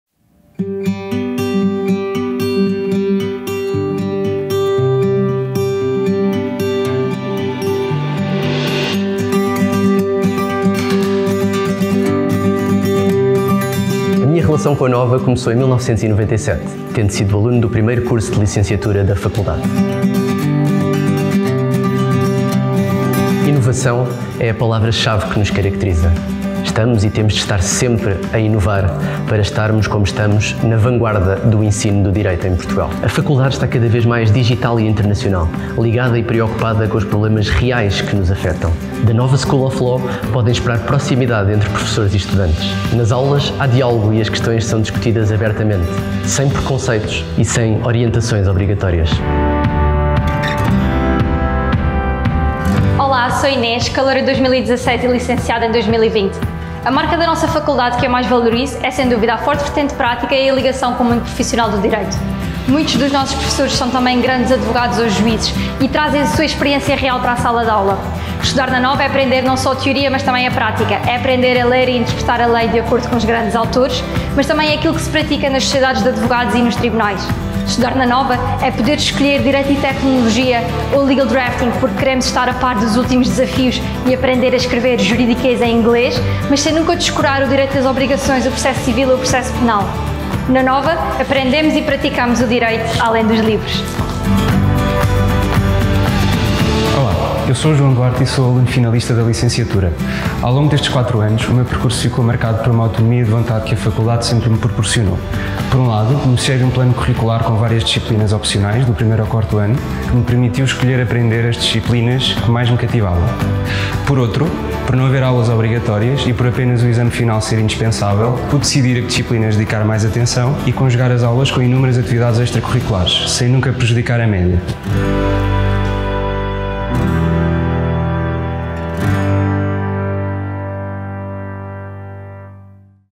Licenciatura em Direito
Inovação. Internacionalização. Proximidade. Diálogo e debate sem preconceitos e orientações obrigatórias. Tudo isto é parte do ADN da NOVA School of Law. Uma Faculdade virada para as realidades que nos afetam, com uma forte componente prática e forte ligação com o mundo profissional. Porque estudar na NOVA School of Law é aprender e praticar o Direito além dos livros. É aprender a mudar o mundo.